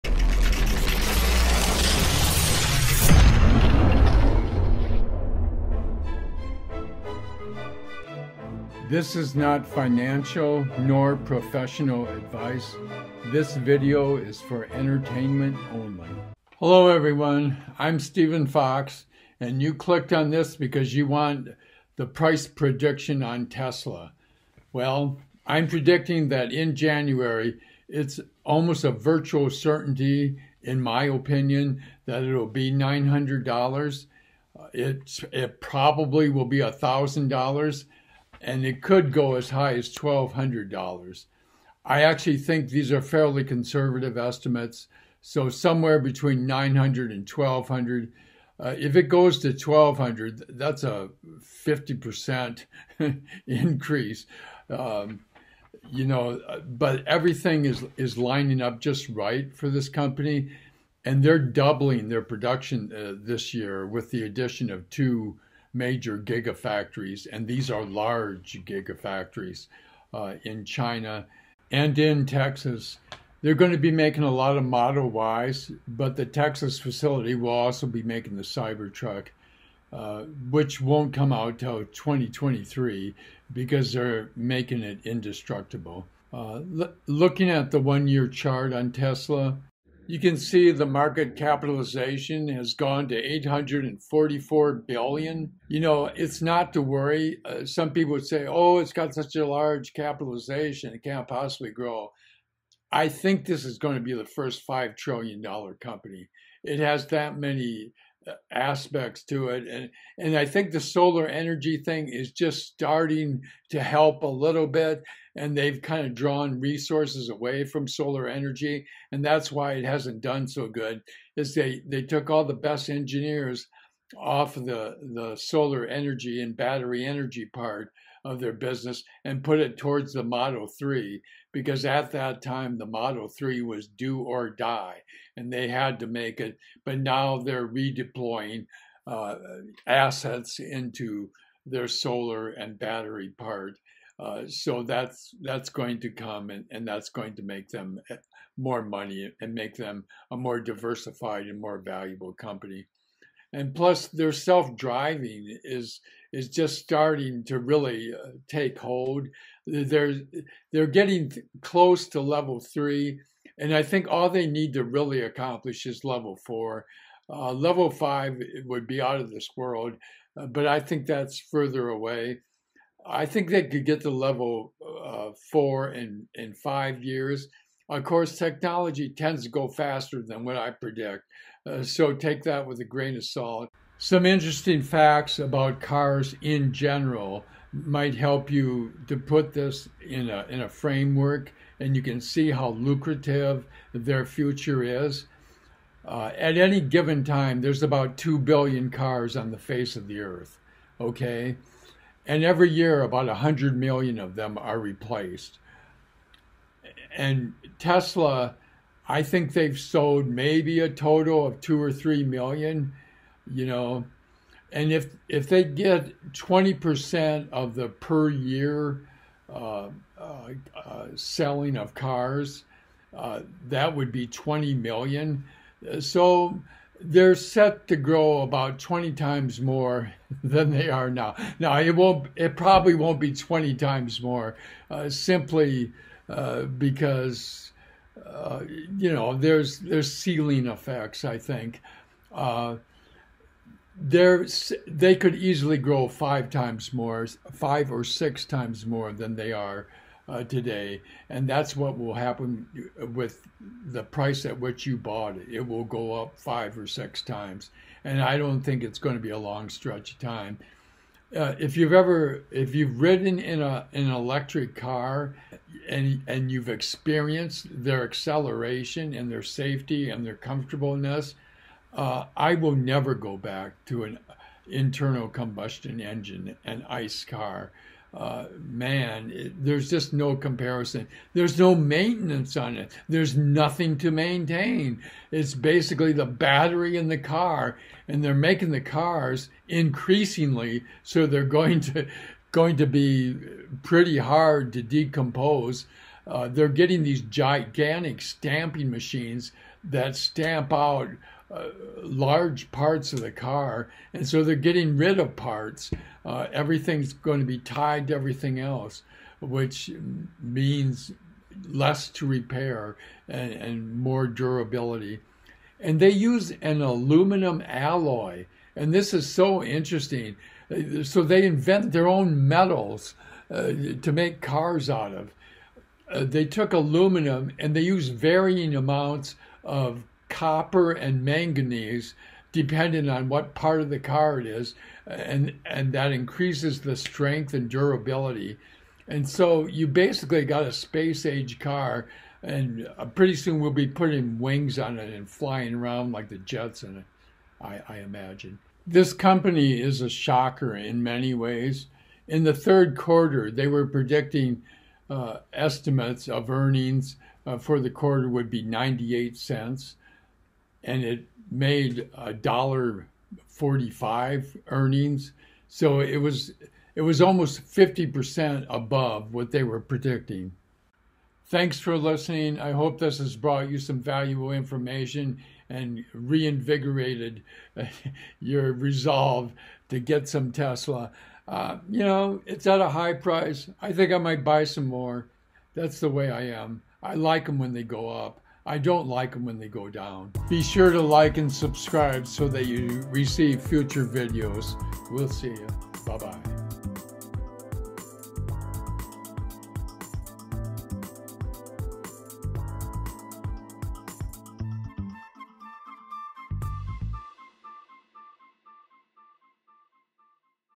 Tesla Price Prediction: Super Hot High Growth Stock
I predict what Tesla's price is likely to be in January of 2022. This over 800 billion company could eventually be the first five trillion dollar market capitalization company. I discuss factors driving the growth of Tesla and electric cars. Tesla's one year stock price chart is explained.